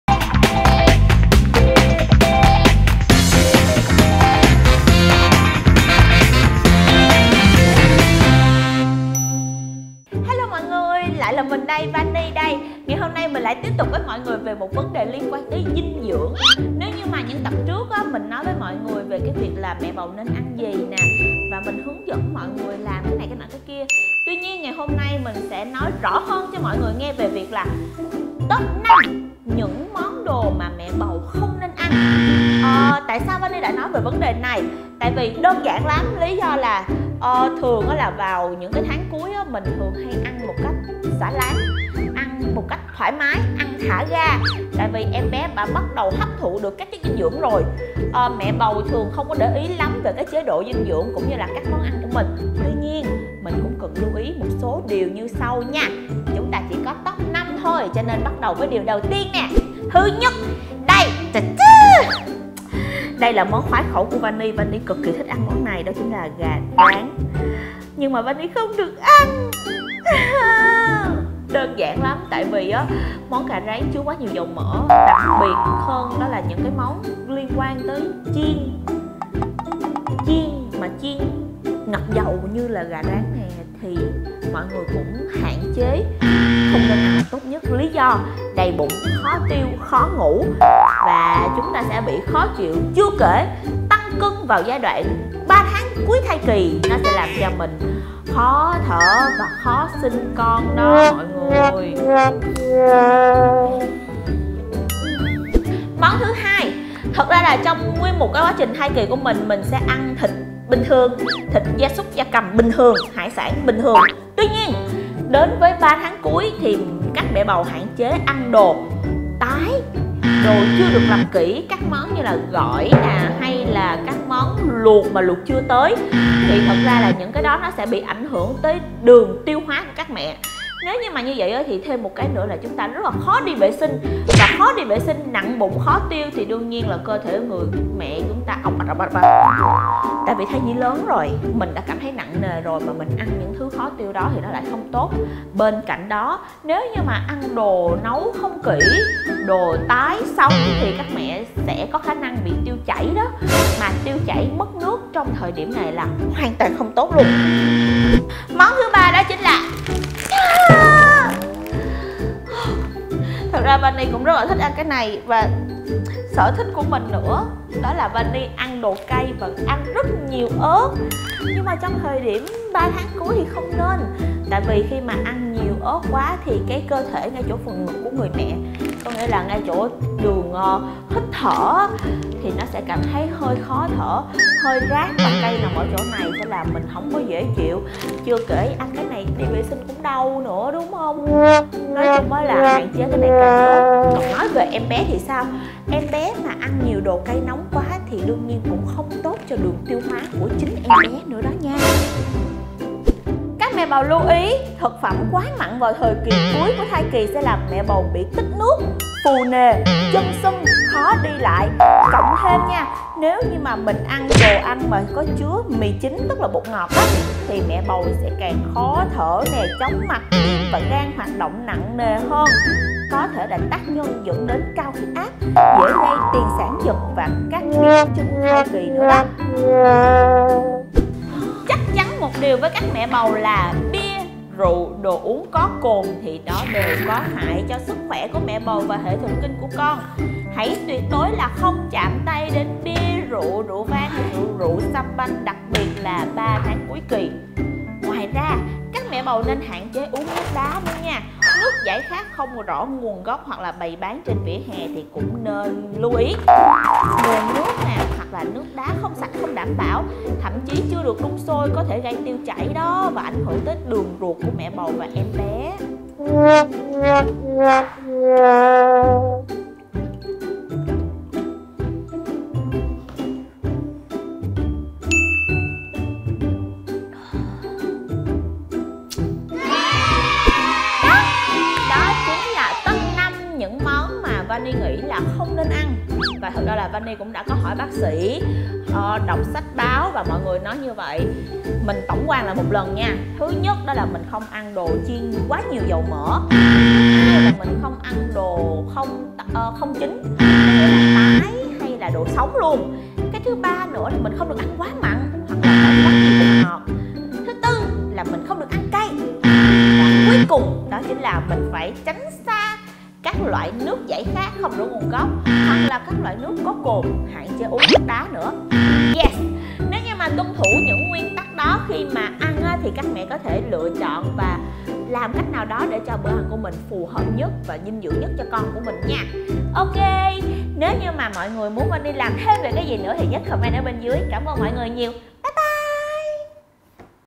TOP 5 ĐỒ ĂN KHÔNG NÊN ĂN VÀO 3 THÁNG CUỐI | MẸ BẦU & DINH DƯỠNG THÔNG MINH | VANNIE TV
Cùng Vannie tránh 5 loại đồ ăn tuyệt đối nên tránh hoặc hạn chế mà mẹ bầu nên để ý nhé.
Comment cho Vannie để mình biết các bạn đang chờ đón những nội dùng nào nhé!